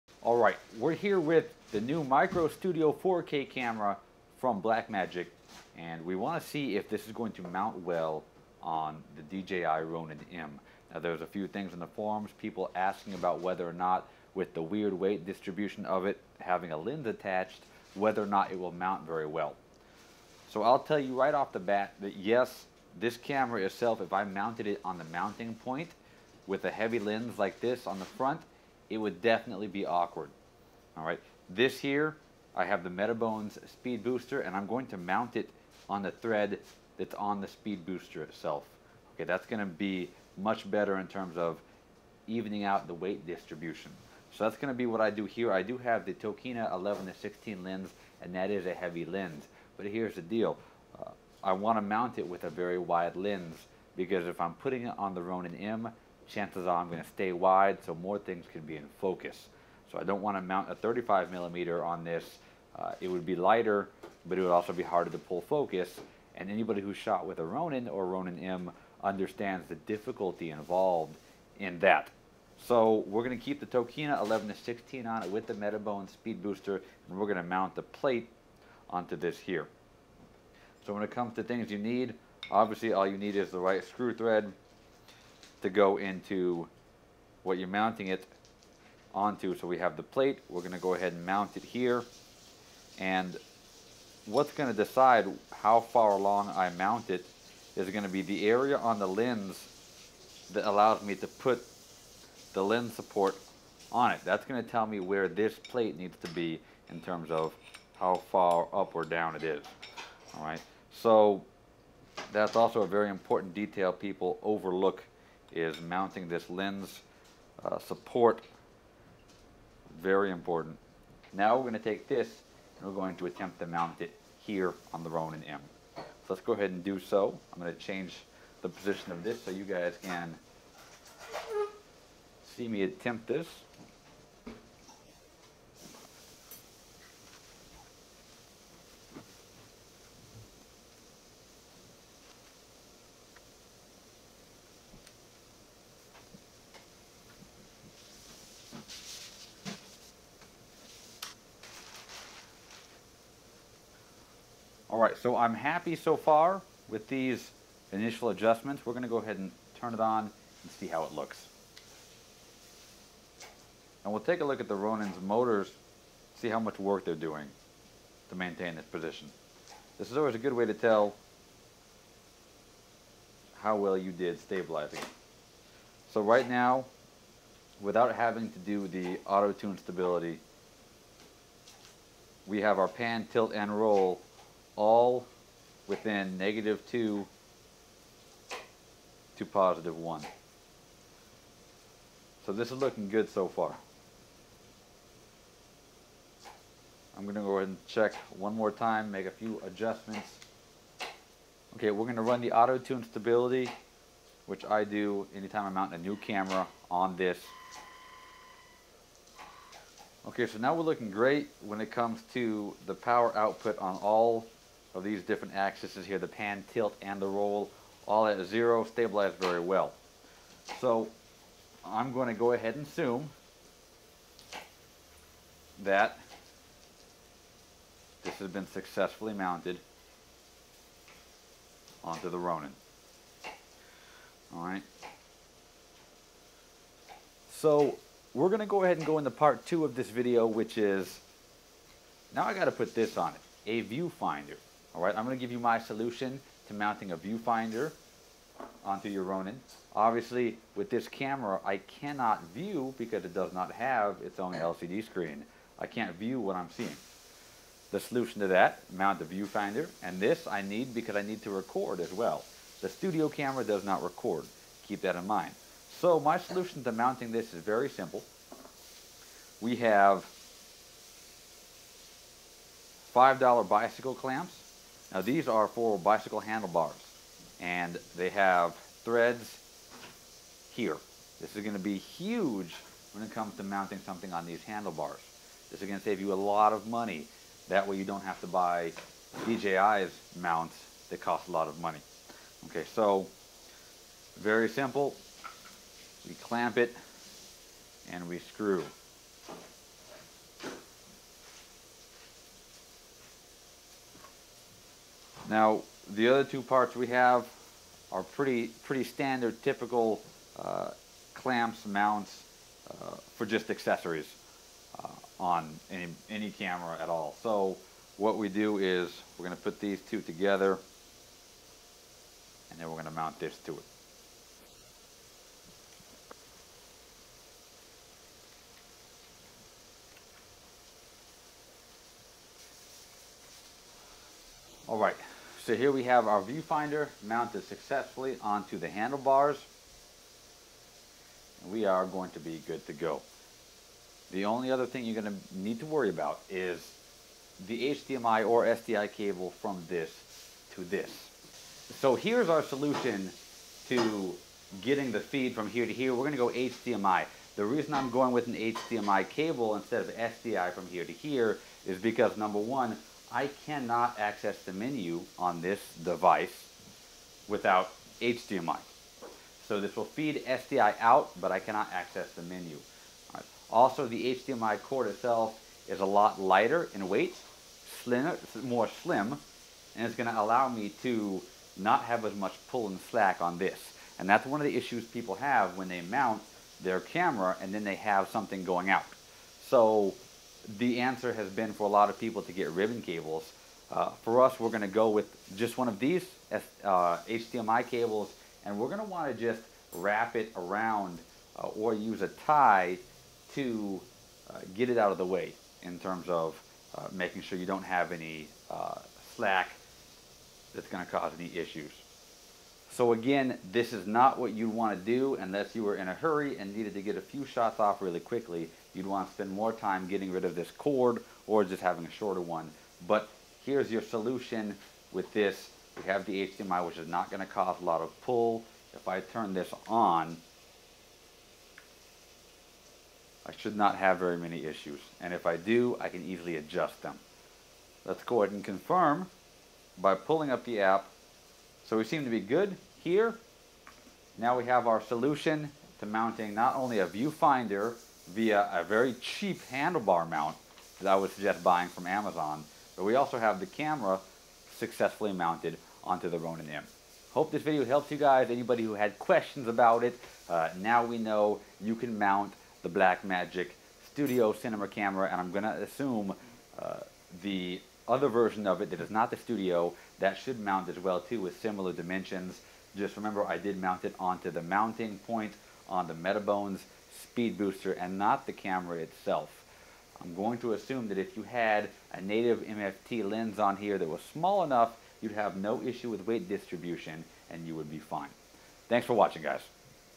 BlackMagic Micro Studio 4k MOUNTED on the DJI Ronin M w/ Video Assist
Successfully mounting the new Black Magic Micro Studio 4k camera on the DJI Ronin M